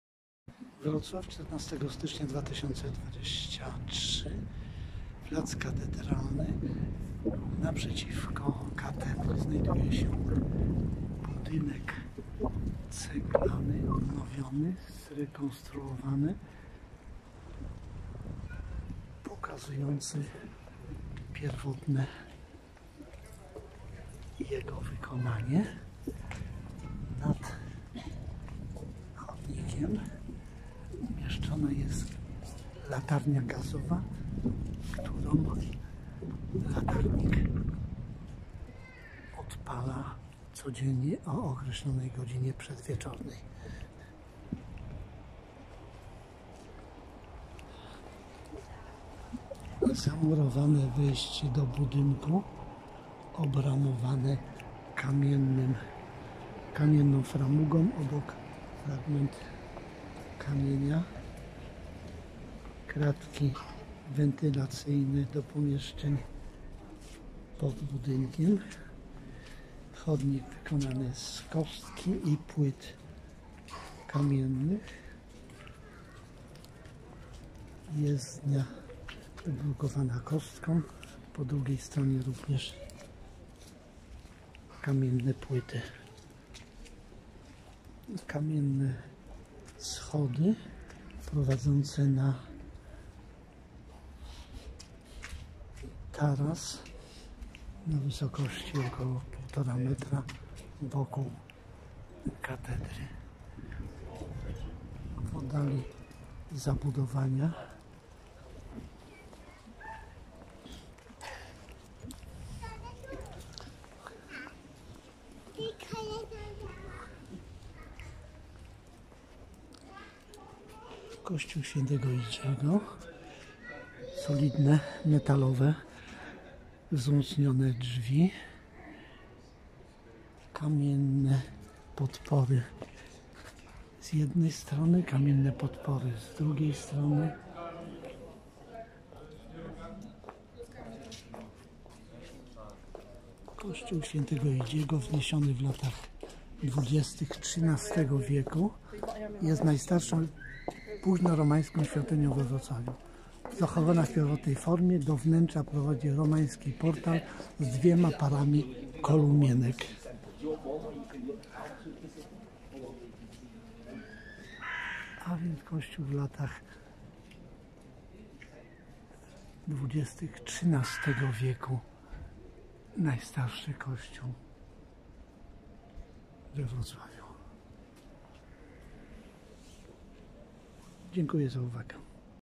Wrocław, Ostrów Tumski, Kościół św. Idziego z XIII wieku VID 20230114 143041
Wrocław, Ostrów Tumski, Kościół św. Idziego z XIII wieku